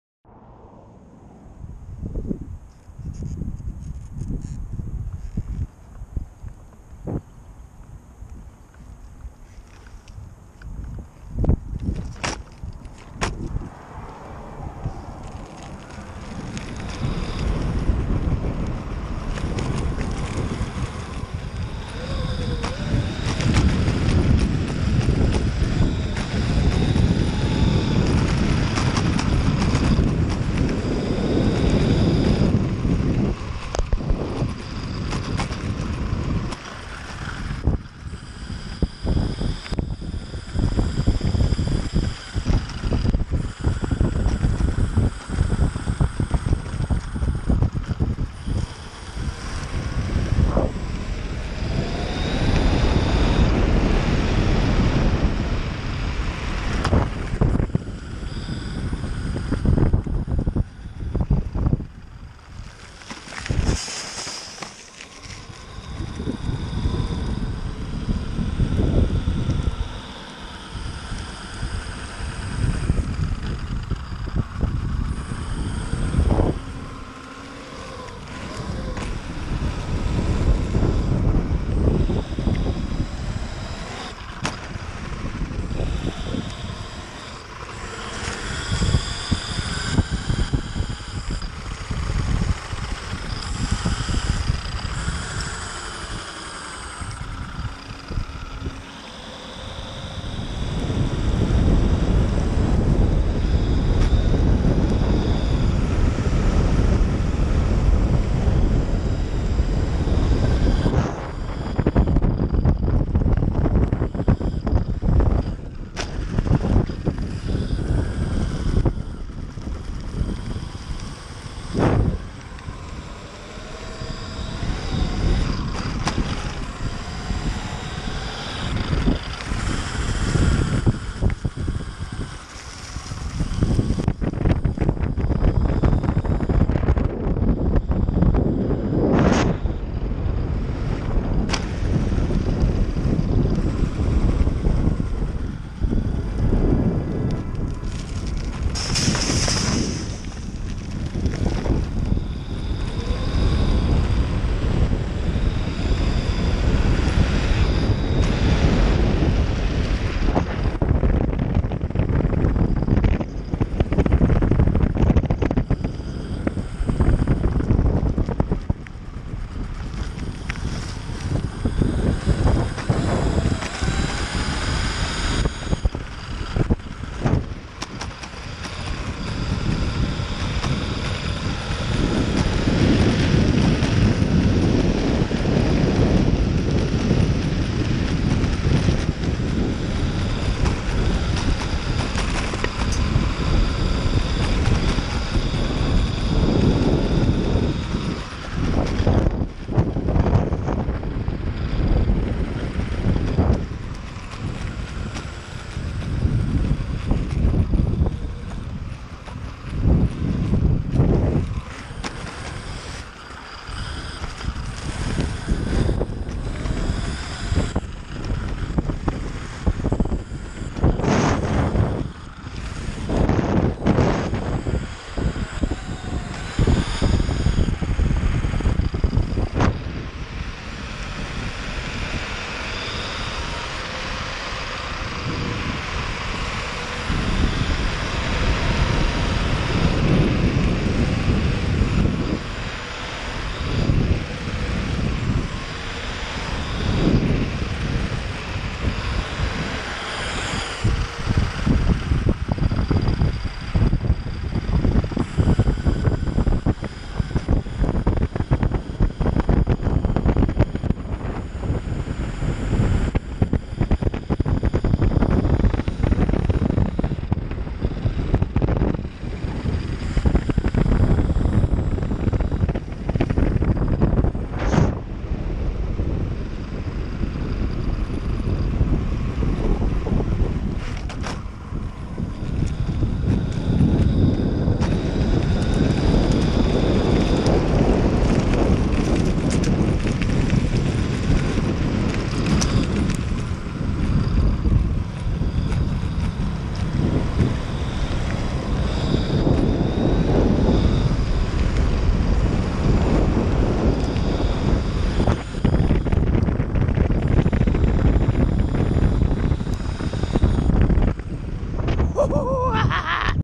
TRAMPA E-BOARD INDUSTRIAL BASH [BREAKING IN ETOXX DRIVES]
Found some quiet industrial area today whilst hitting the red paths. Gotta love bike paths never know where they might take u.

The board needs a little work but it is running.. the etoxx gears are great very strong.

MY SETUP:

-TWIN 136kv Trampa Motors

-TWIN Vesc 6.4

-TWIN *E-Toxx* maxi helical gear drive 1:5

-Monster Box

-TWIN 12s Battery (4x 6s Zippy compact 6200mah 40c)

-Electric Holypro deck

-Superstar Hubs

-MBS Matrix II trucks (metal base)

(BATS RECOMMENDED ON TRAMPAS SITE)

More fun to come. It dont end here mate...